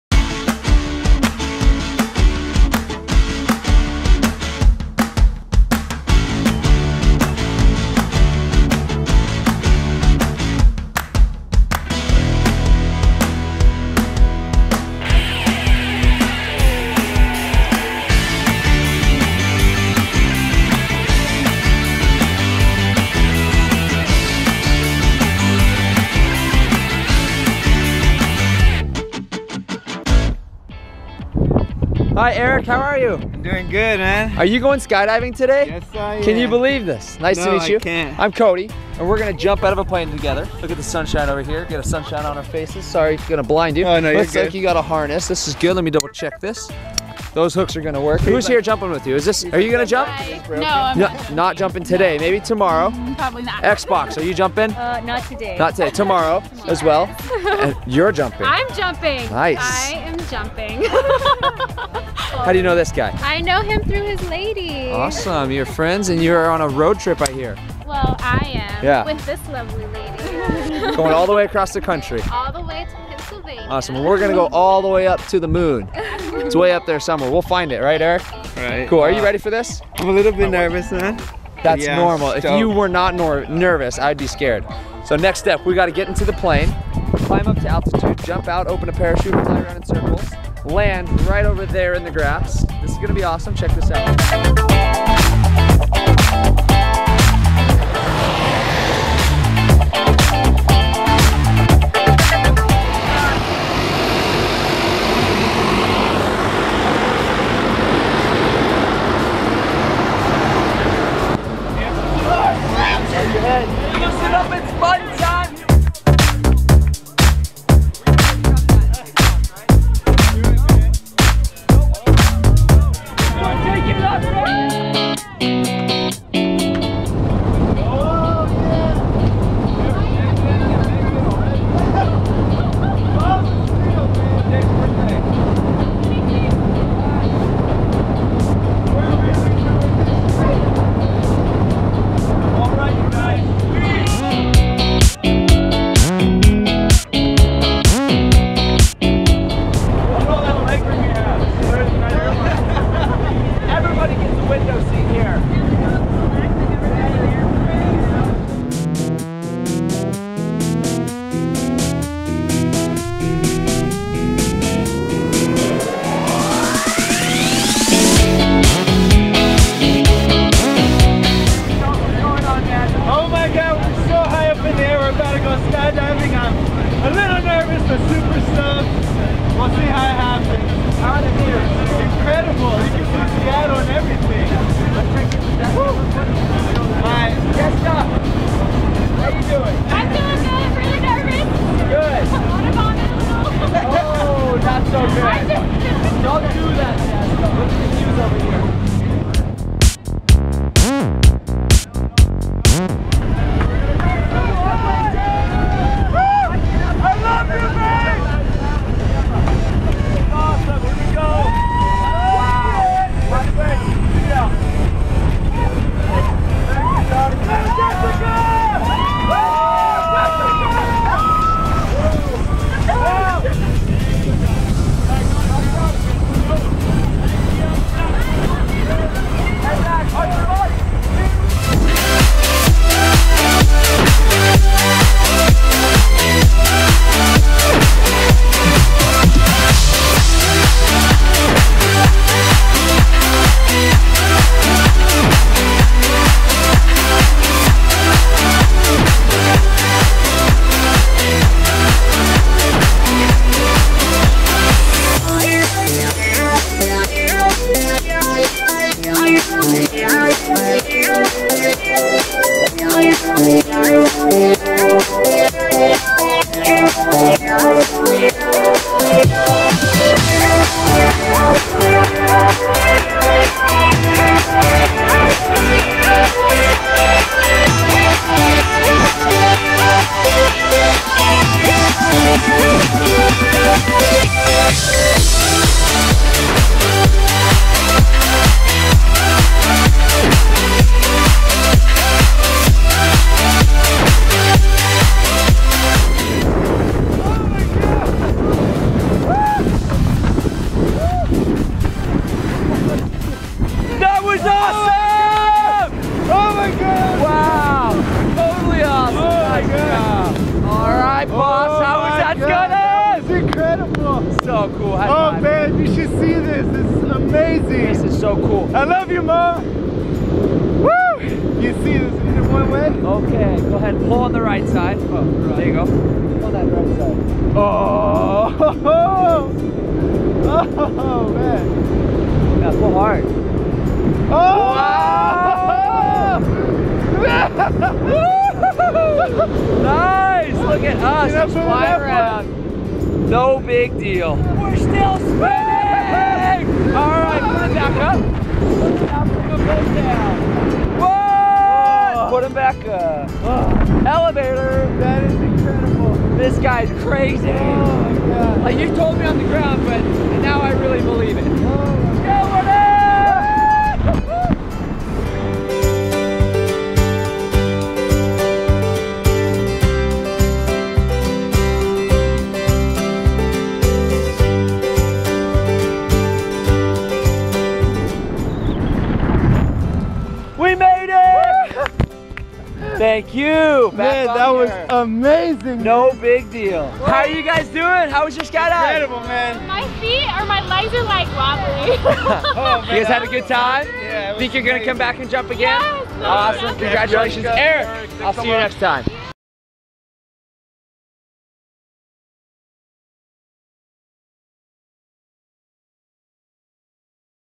Erick Alfaro's Tandem skydive!
Erick Alfaro's tandem skydive at Skydive Snohomish!